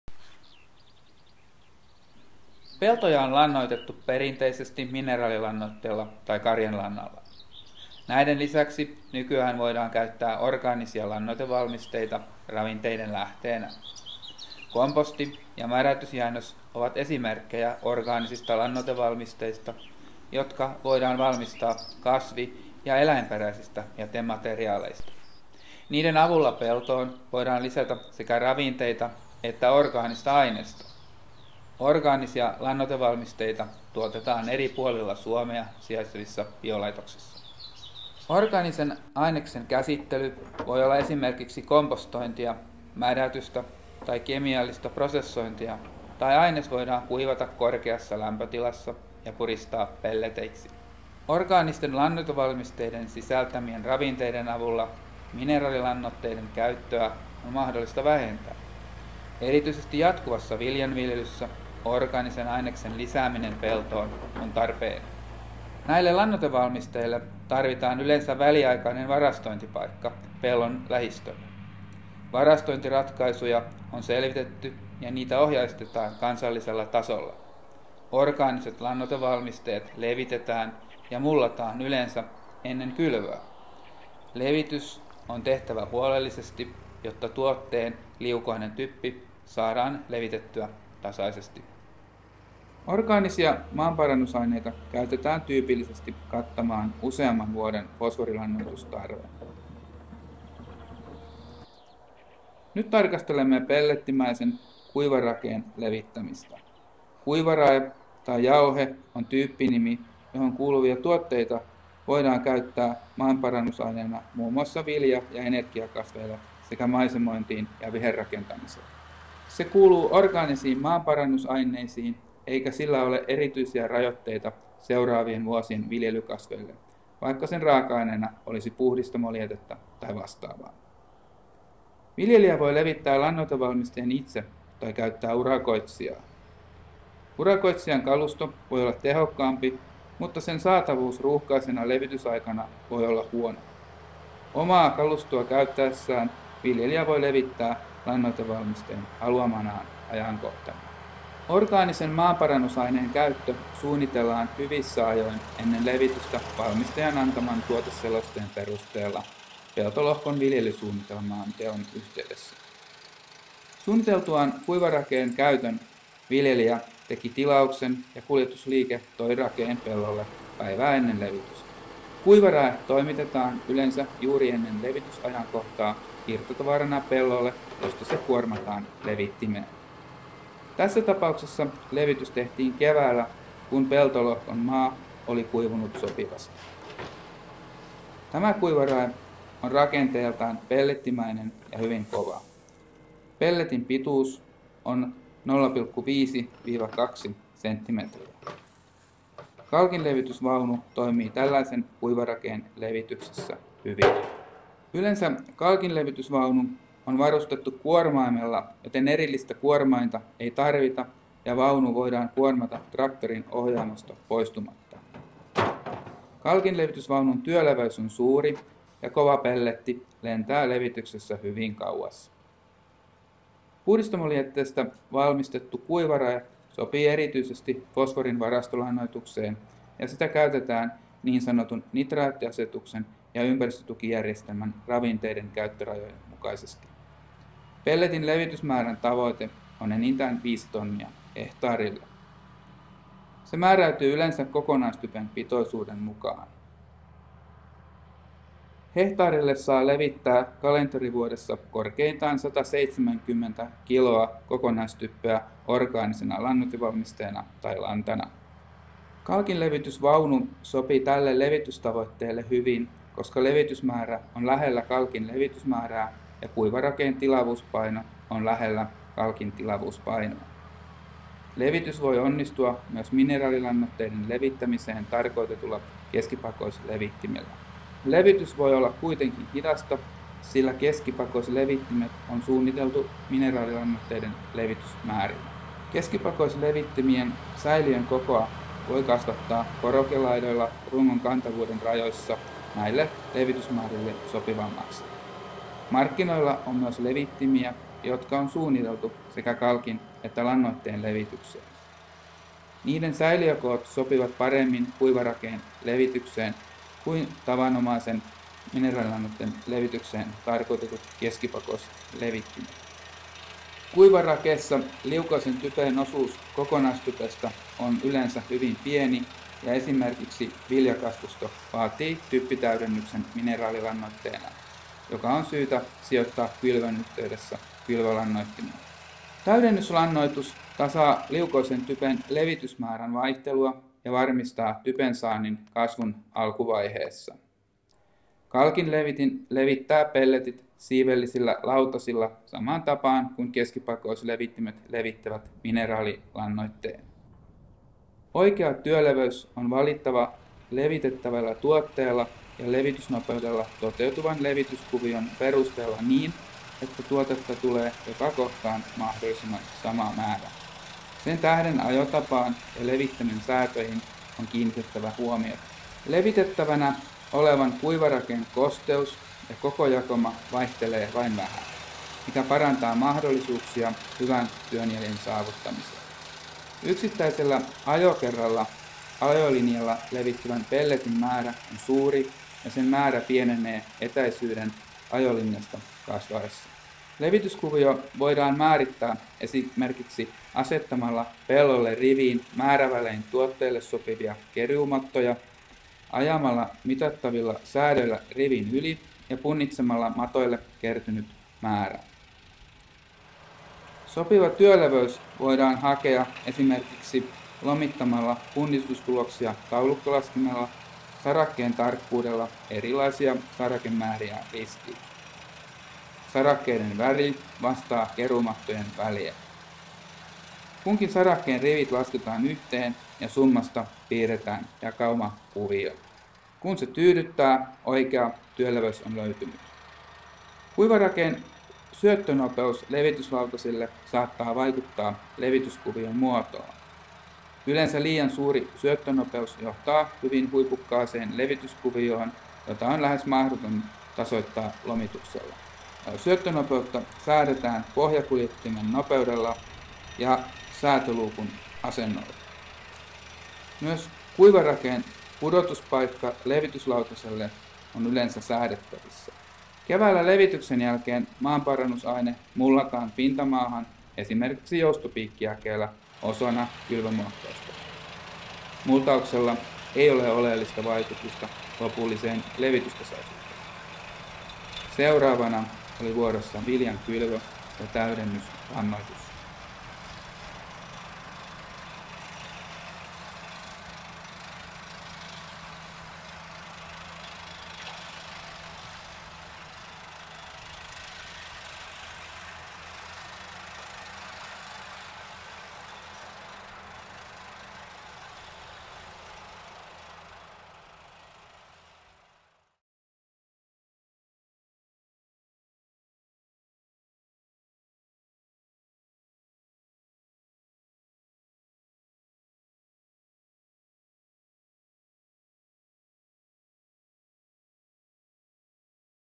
Orgaanisten lannoitevalmisteiden levitys ja käytönsuunnittelu - Kuivarae
Orgaanisia lannoitevalmisteita tuotetaan kasvi- ja eläinperäisistä jätemateriaaleista ja niitä voidaan käyttää viljelyssä mineraalilannoitteiden ja karjanlannan sijaan. Tässä videossa kerrotaan pellettimäisen kuivarakeen levityksestä.

Video on tuotettu MTT:n hankkeessa LeviLogi – Orgaaniset lannoitevalmisteet lähialueiden tilojen käyttöön. Lisätietoja